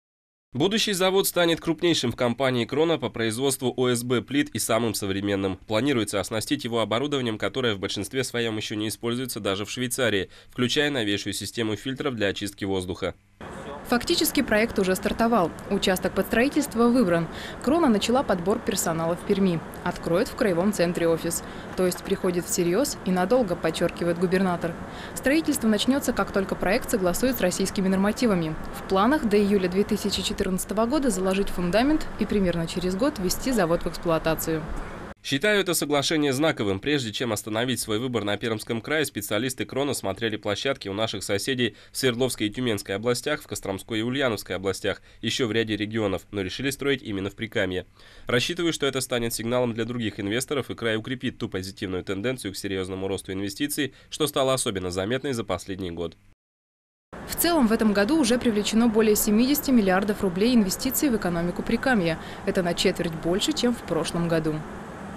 Соглашение Прикамья и швейцарской "Krono Group"
Нерядовое событие. Губернатор Виктор Басаргин и швейцарская "Krono Group" подписали соглашение о строительстве крупного деревообрабатывающего производства в Пермском крае. Об этом глава региона сообщает в своем интернет-блоге. За два - два с половиной года в экономику нашего региона инвестор вложит порядка двухсот миллионов евро. В Краснокамском районе "Кроно Group" построит завод по выпуску ОСБ-плит с проектной мощностью более шестисот тысяч кубометров в год. Продукцию будут поставлять на российский и японский рынки.